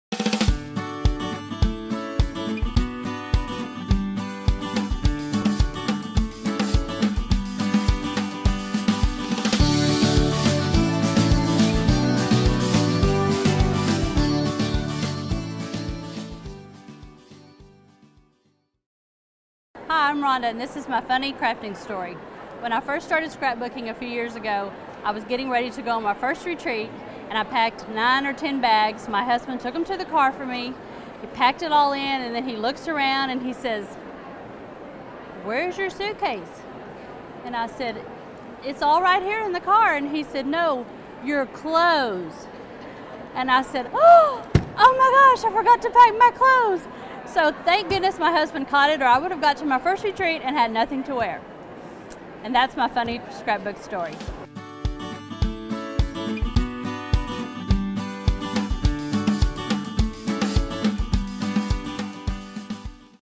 The Naked Scrapbooker?
She was all packed up to go to the Scrapbooking Retreat, but . . .
We filmed guests at our Grapevine TX Scrapbook Expo sharing their favorite crafting stories:
My Shocking Crafting Confession
Most Touching Crafting Experience
Funniest Crafting Story
Worst Crafting Injury
Enjoy this series and start thinking about YOUR favorite crafting story to share!